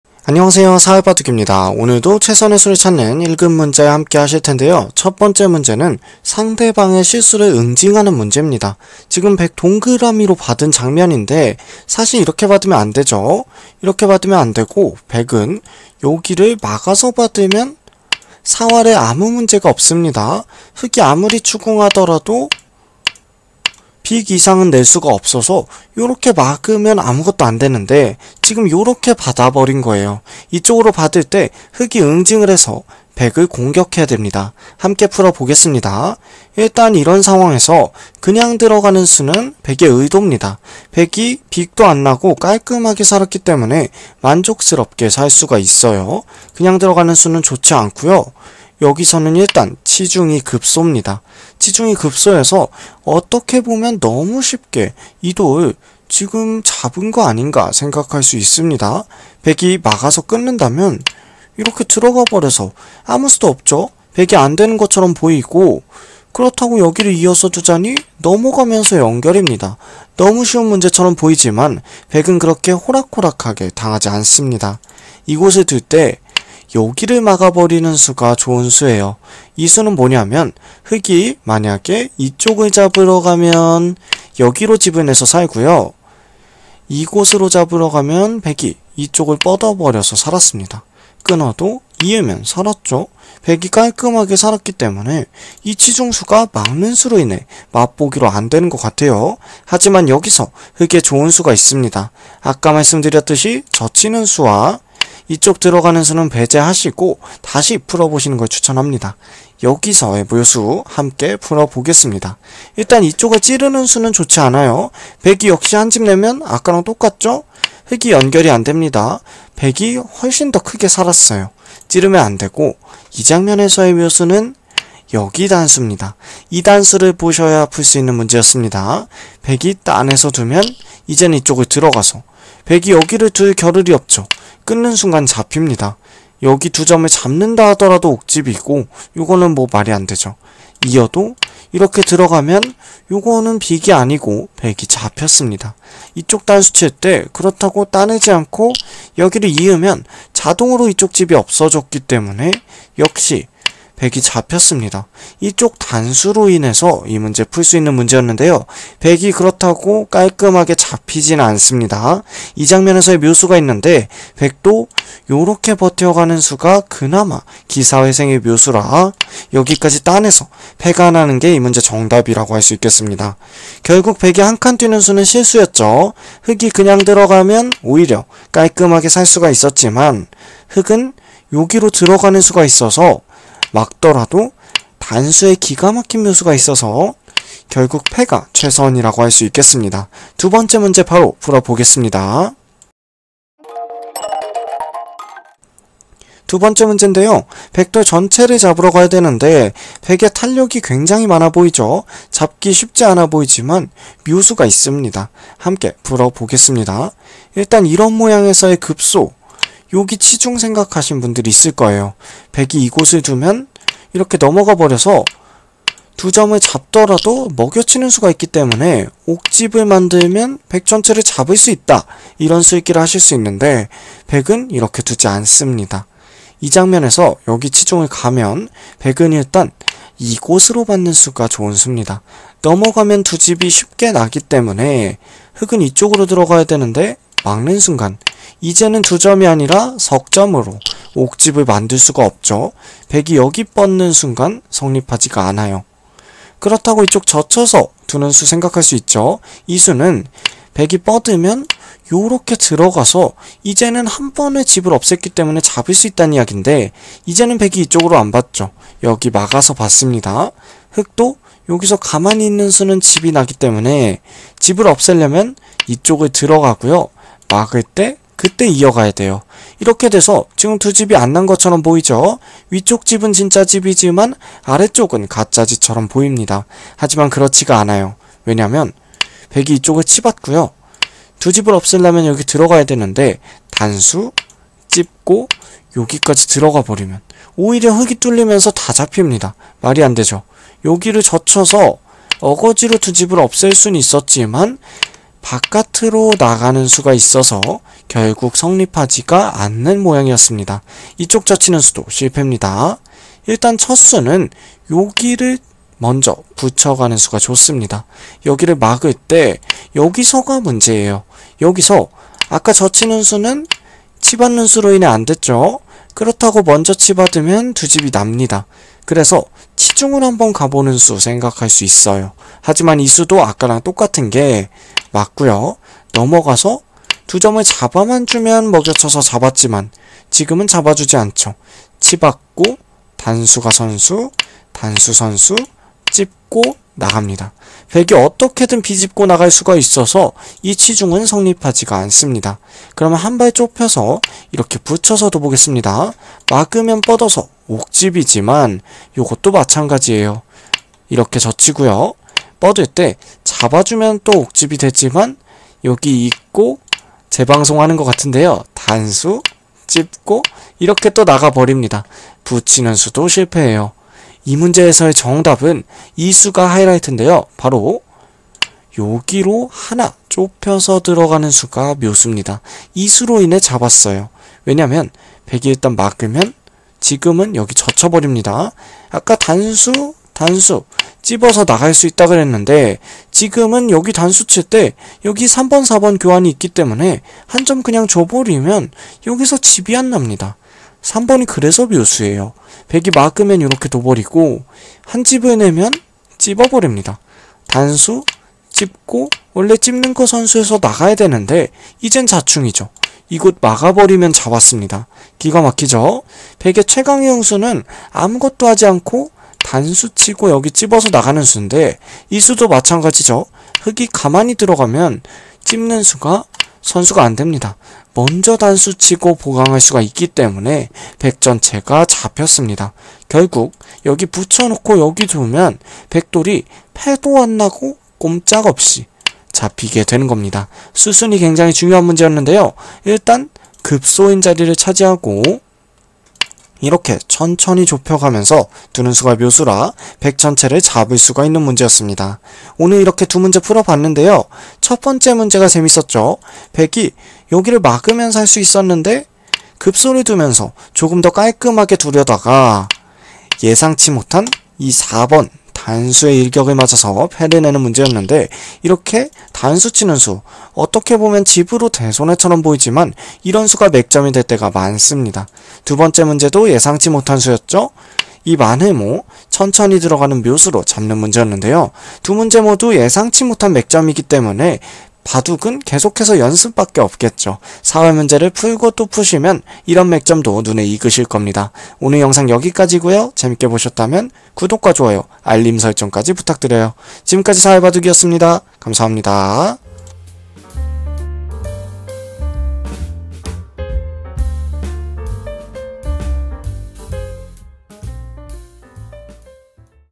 사활 바둑 묘수 - 상대방의 실수를 응징!! 백을 공격하세요!
아무리 유리한 바둑도 한 번의 수읽기 착오로 패배하게 됩니다.
오늘부터 저희 [사활바둑]을 통해 수읽기의 힘을 키워보세요!